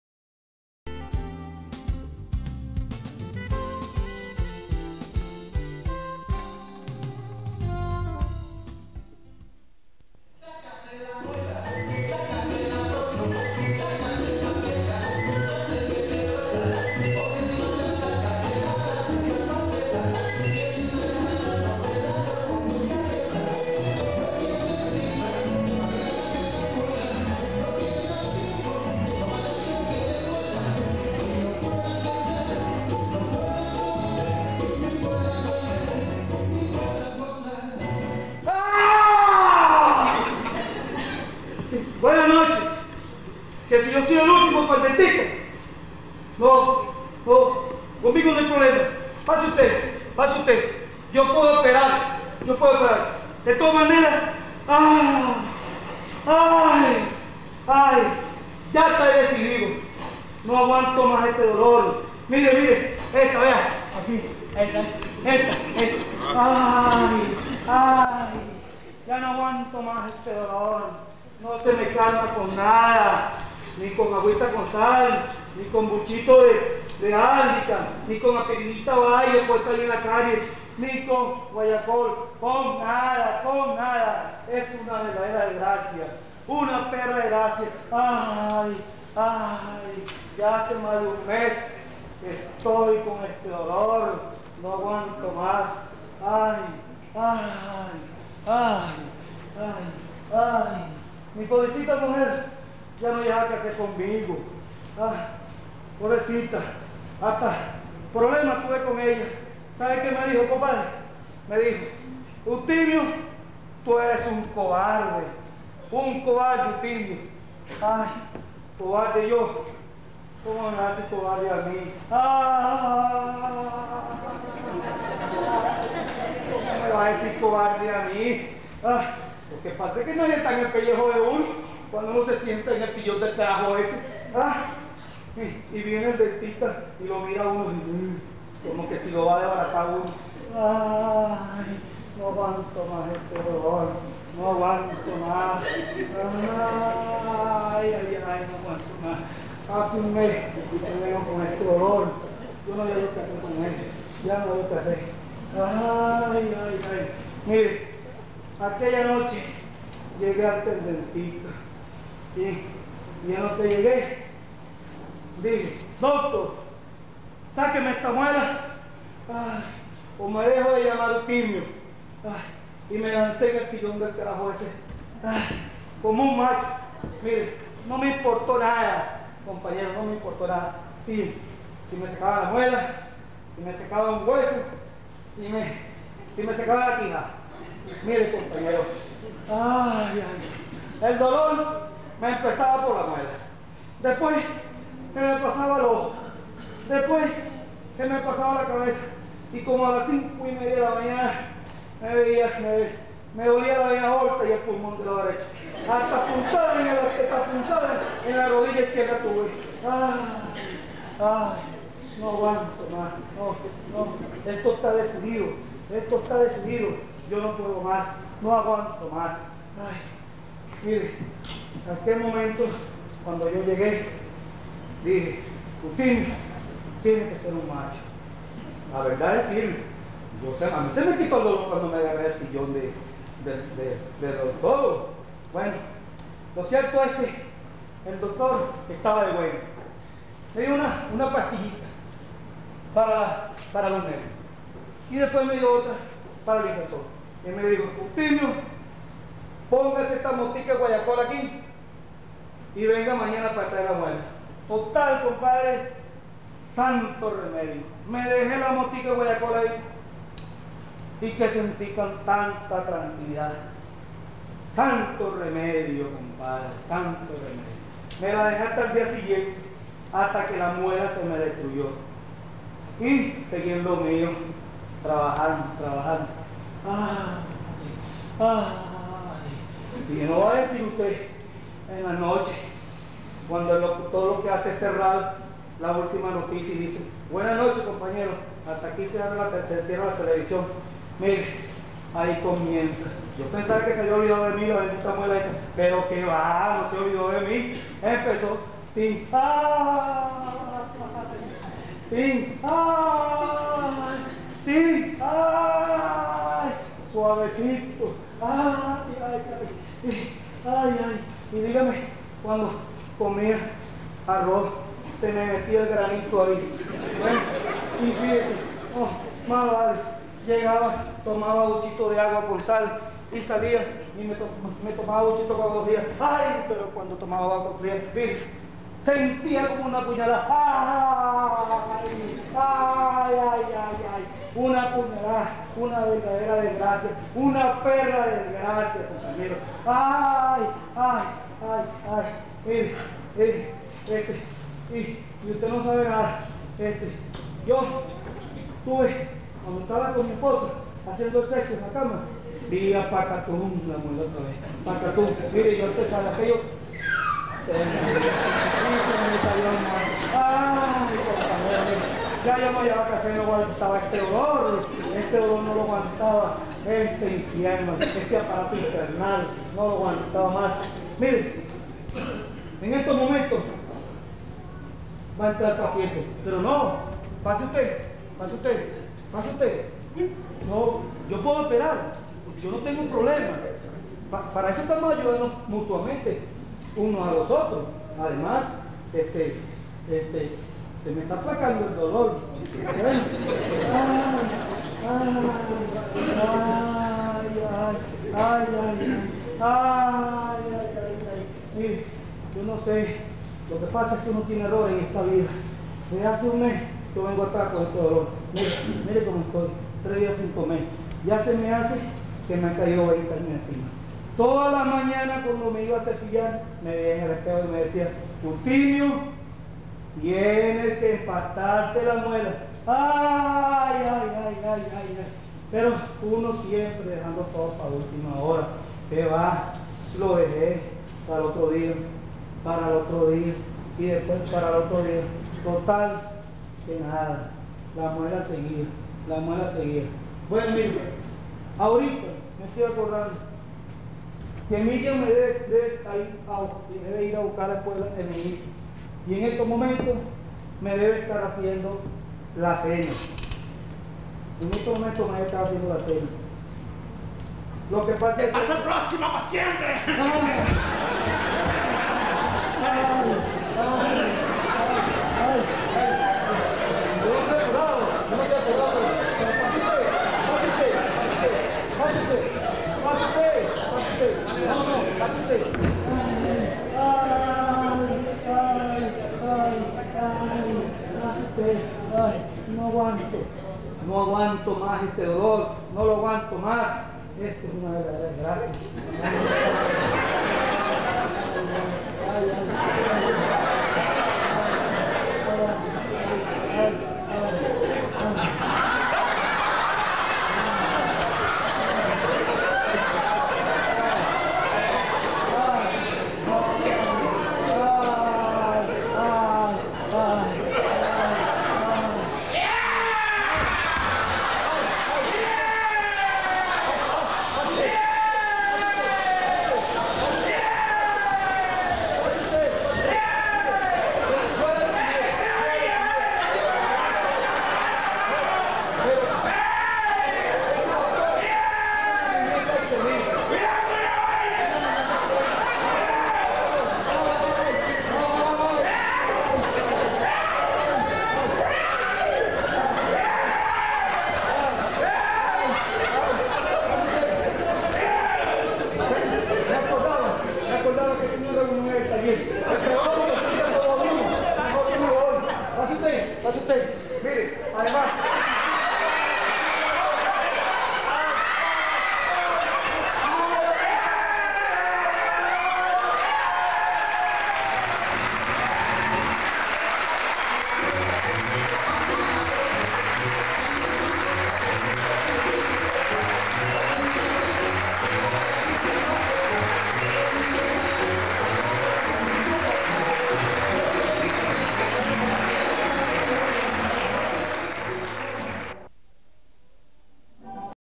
Obra Sácate la muela 14/12/1995
Grabado por  Sigfredo Romero Sánchez
Motivo: II Encuentro de Teatro Inter-gerencias
Corpoven, Refinería el Palito
14 de diciembre de 1995.